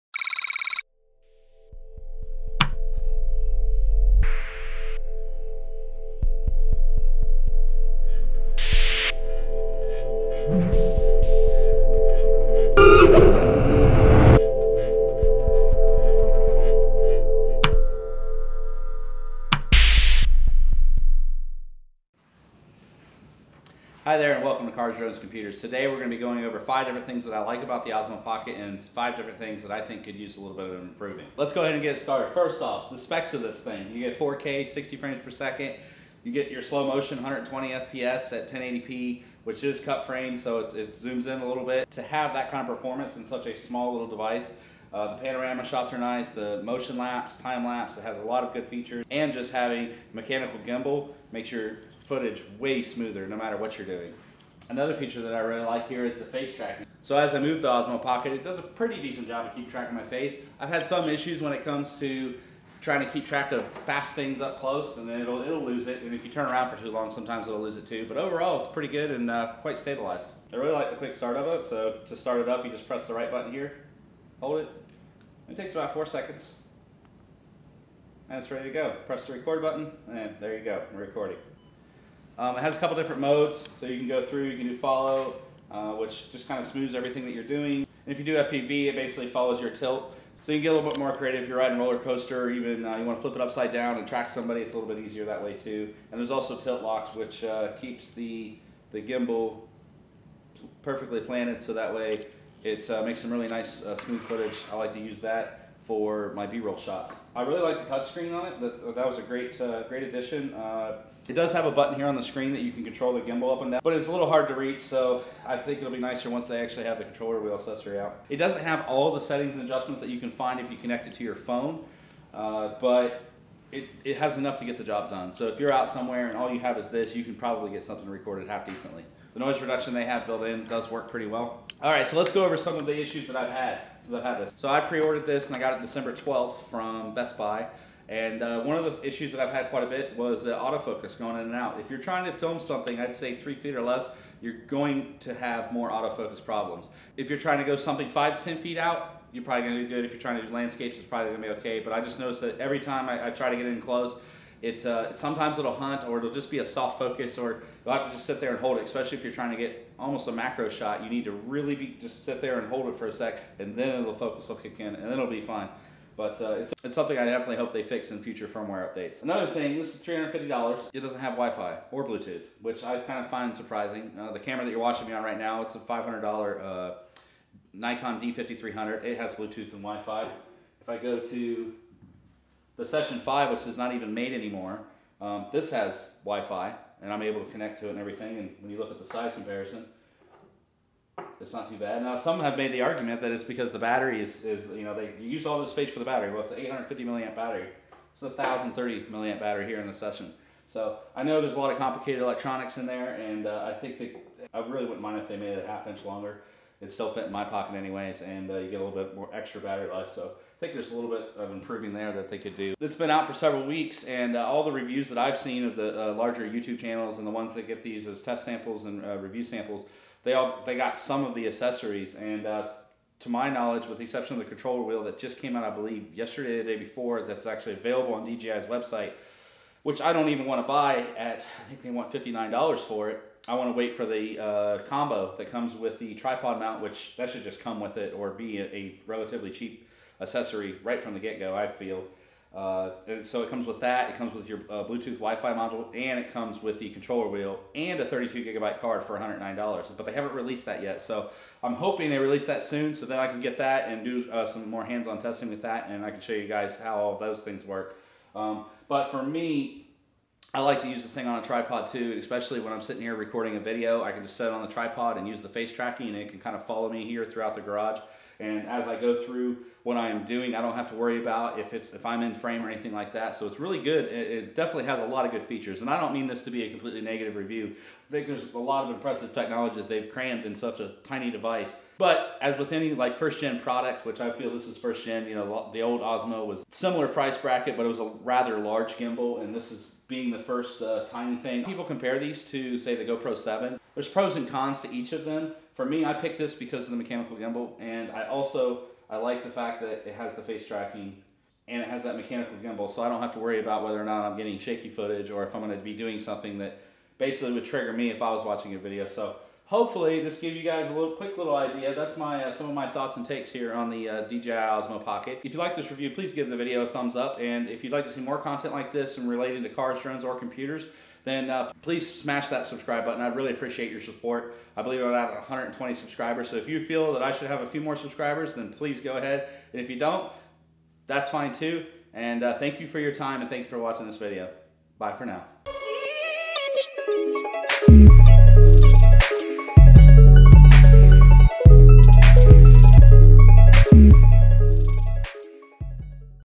DJI Osmo Pocket Review
Here is a quick review on the pros and cons of the Osmo pocket. These are my personal opinions!